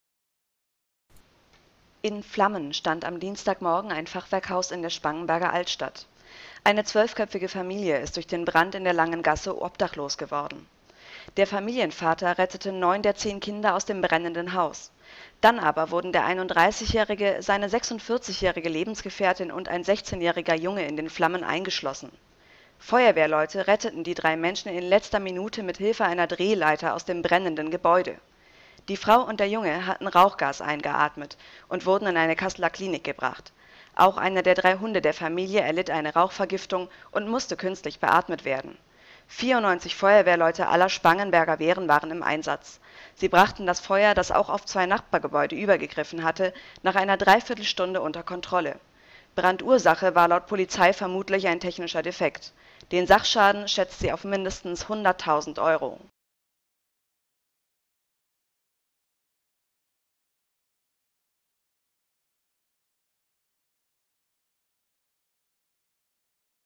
Fachwerkhaus in Spangenberg brennt nieder: Familie obdachlos
Spangenberg, 16. Juli 2013: Fachwerkhaus in Spangenberg brennt nieder: Familie obdachlos - Aus einem brennenden Haus in der Spangenberger Altstadt haben Einsatzkräfte am Dienstag drei Menschen gerettet. Das Feuer griff auf zwei benachbarte Gebäude über.